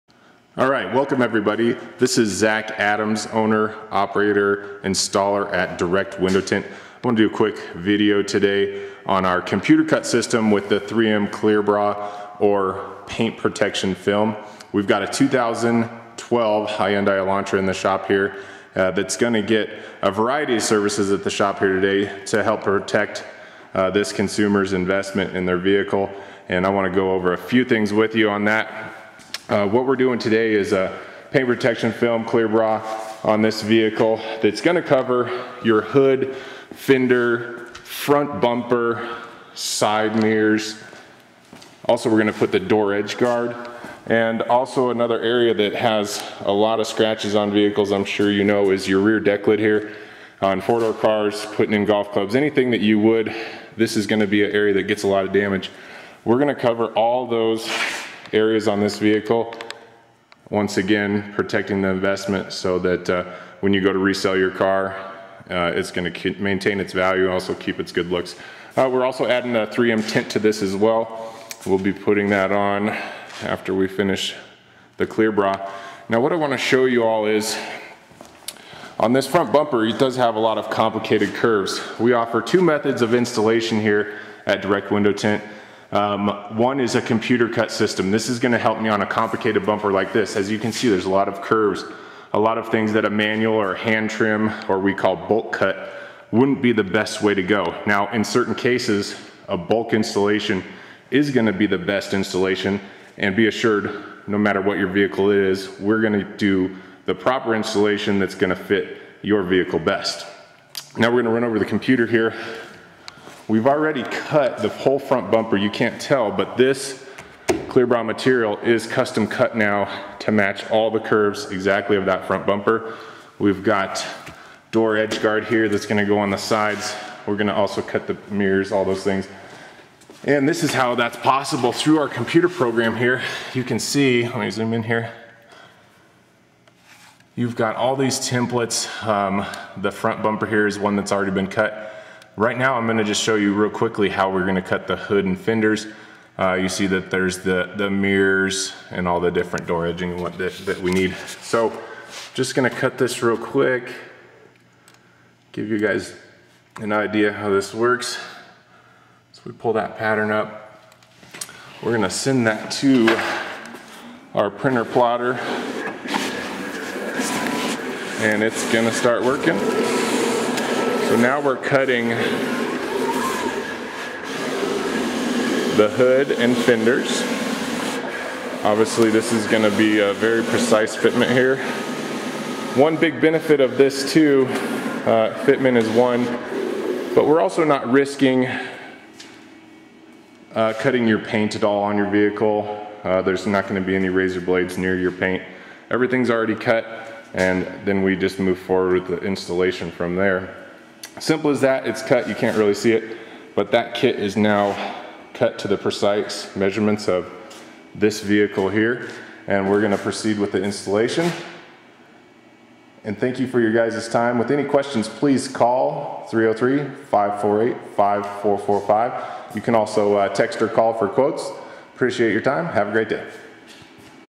3mclearbracutting.MOD
features benifits and cutting of 3m clear bra paint protection film